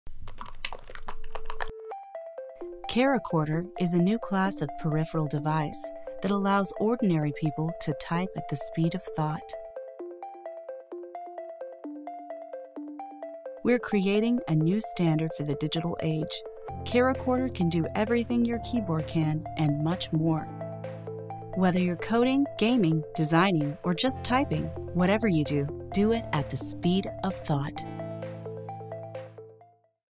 CharaChorder - Type At The Speed Of Thought™
With CharaChorder, users can type characters just like an ordinary keyboard or they can press many letters simultaneously to output entire words in a single motion. This allows for typing faster than physically possible on any other keyboard.